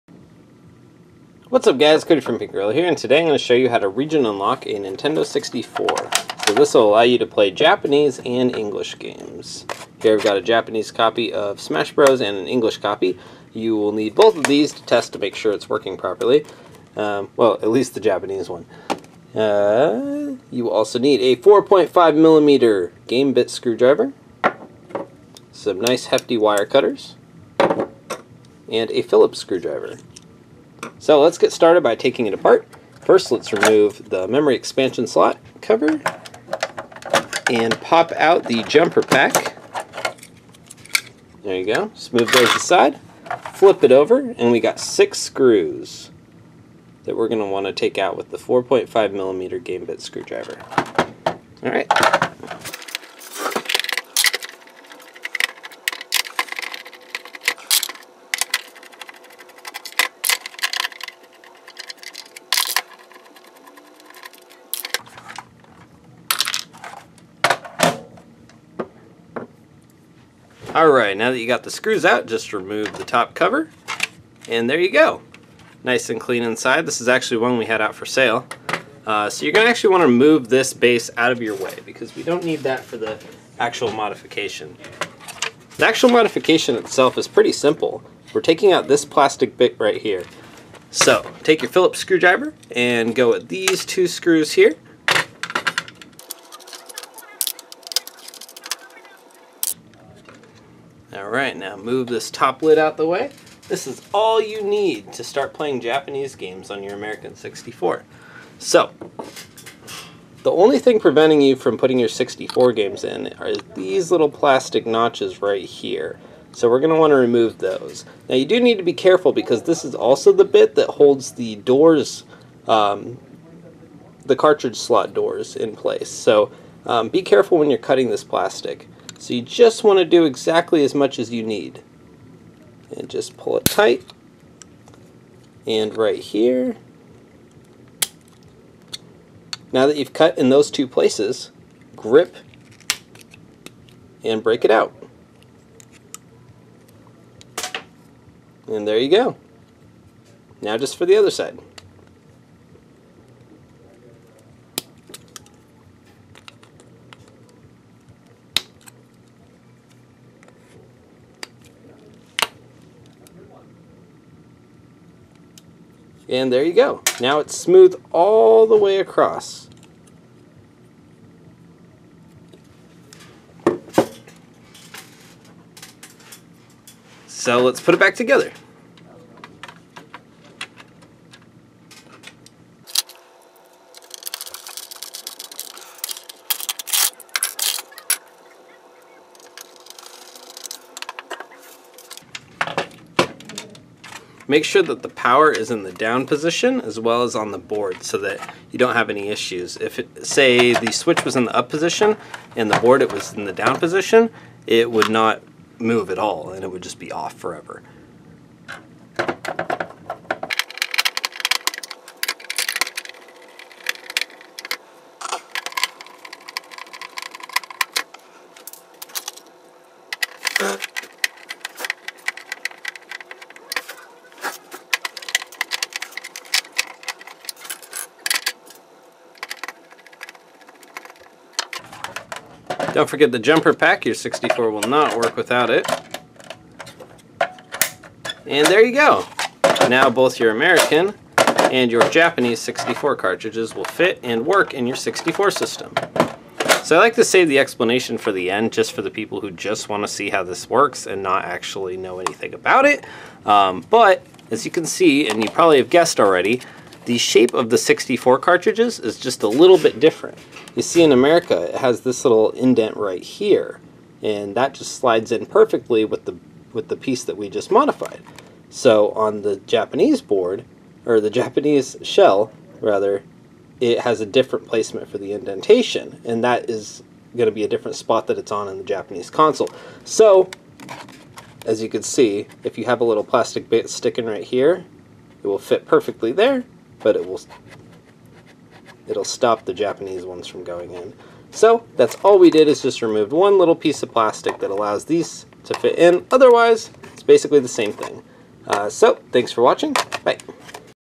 How to Region Unlock a N64
Materials Required:
4.5mm Gamebit Screwdriver
Phillips Screwdriver
Wire Cutters

We offer this service in store for free with the purchase of a Japanese N64 game or $5.